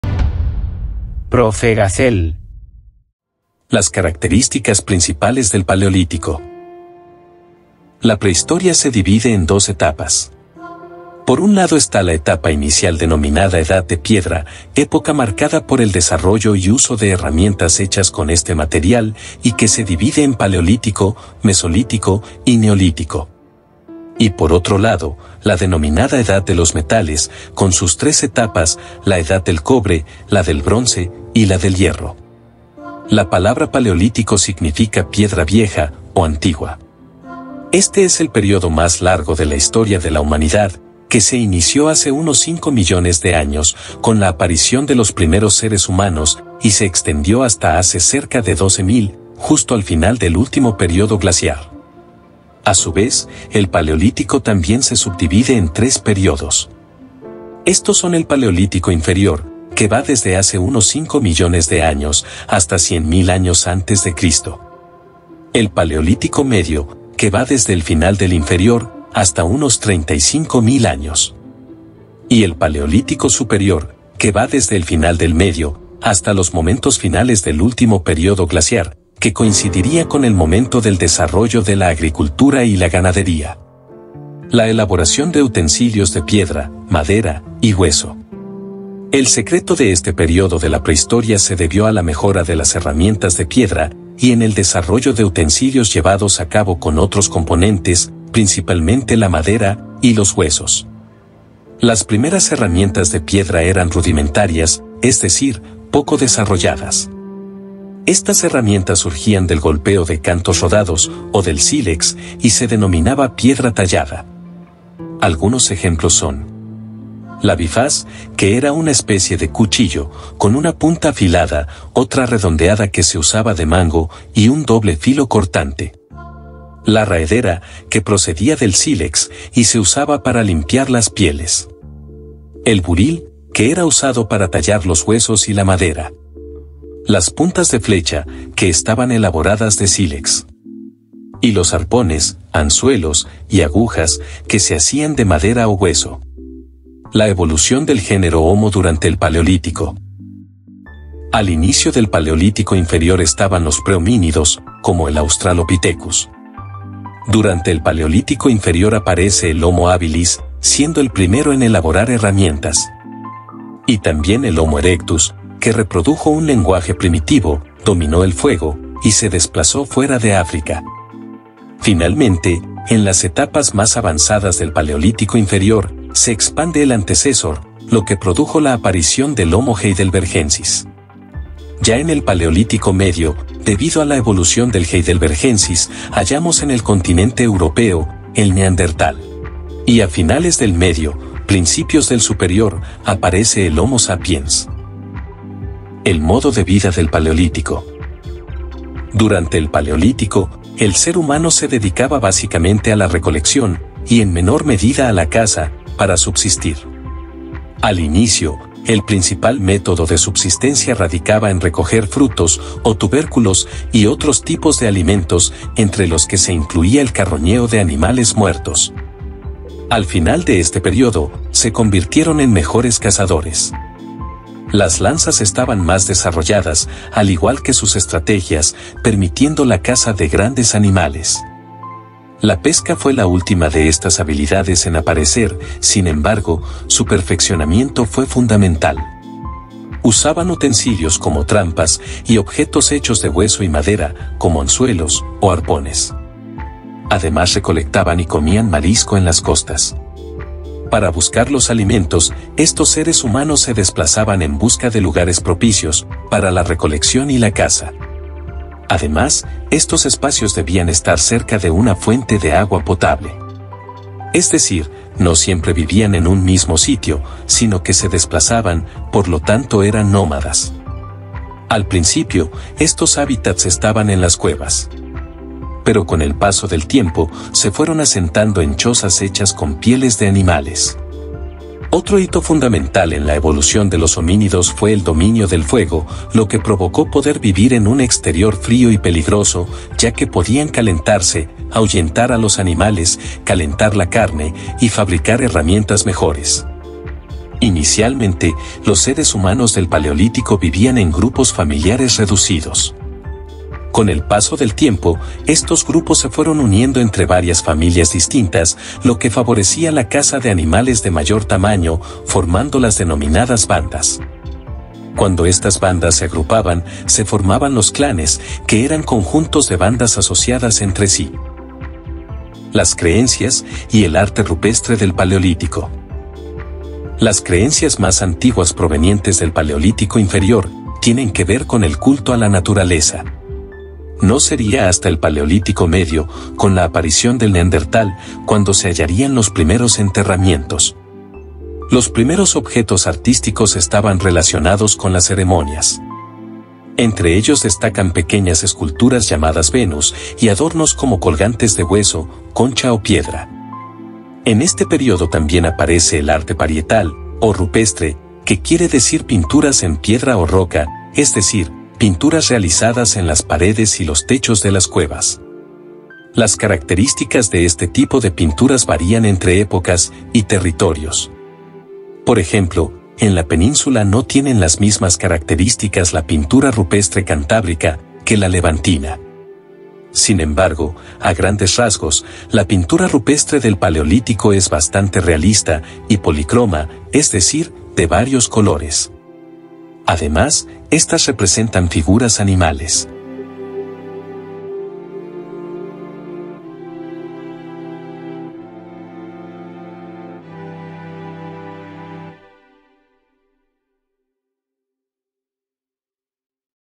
El paleolítico y la tecnología de las primeras herramientas
Vídeo explicativo de la etapa de la Prehistoria denominada Paleolítico, y de sus principales características (definición, modo de vida, herramientas o arte).